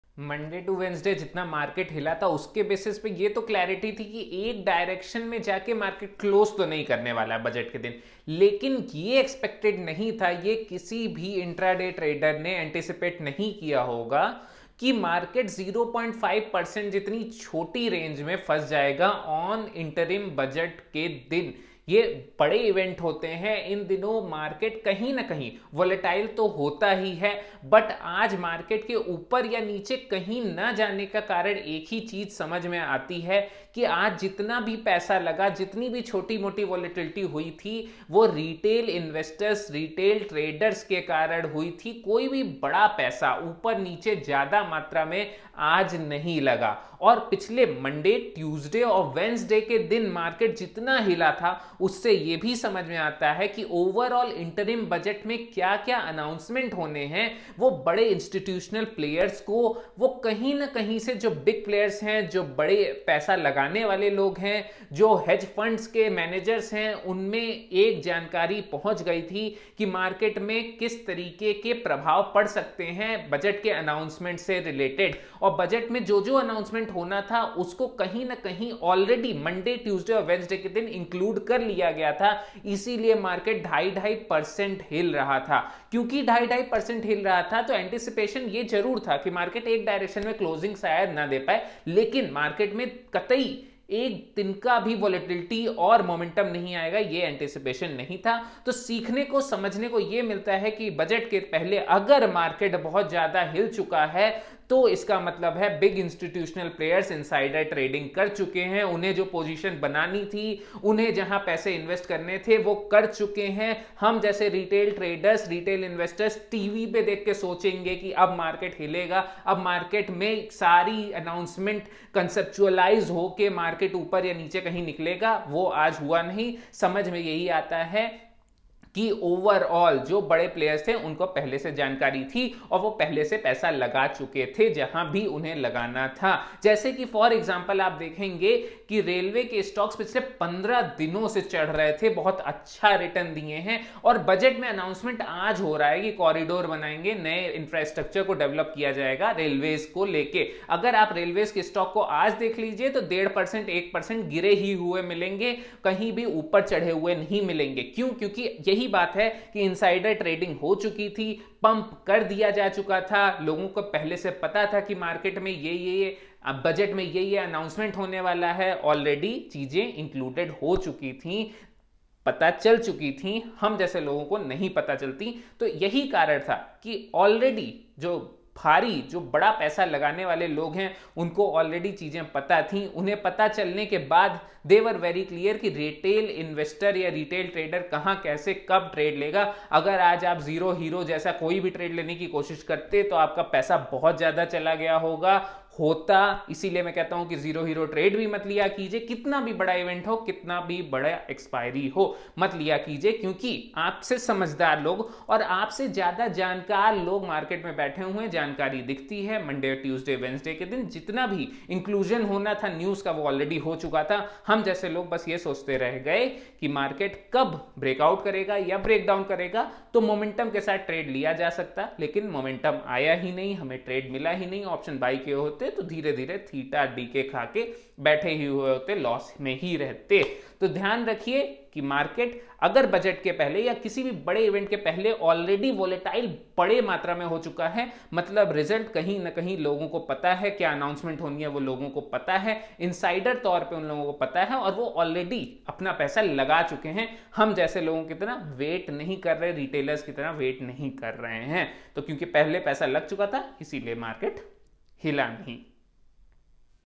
Why Market did not move on Interim Budget Day 2024?
In the video I will be talking about why the Indian Stock Market Indices like Nifty 50, Bank  Nifty, Sensex and MidCap Nifty did not move more than 0.5 percent even on such a major event of Interim Budget 2024. Also discussed what a beginner and a new intraday trader of Indian share market can learn from trading on such days and how zero hero trades should not be planned for these days.

Narration By- Kunvar Aishwary
Narration Language - Hindi
Geographical Relevance - India
Profession Relevance - Intraday Traders of Indian Share Bazaar